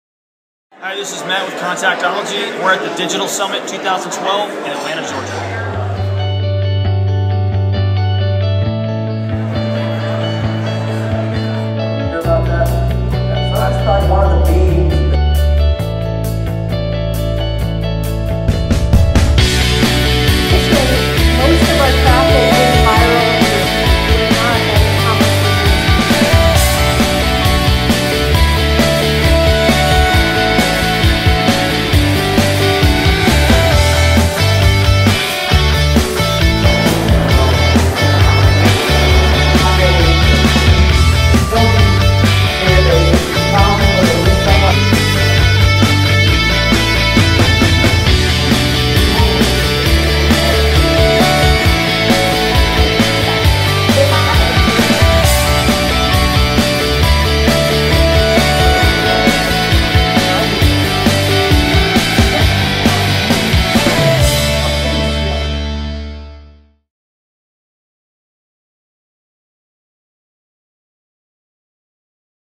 Contactology at the Digital Summit 2012
Contactology is proud to be a sponsor of the second annual Digital Summit in Atlanta.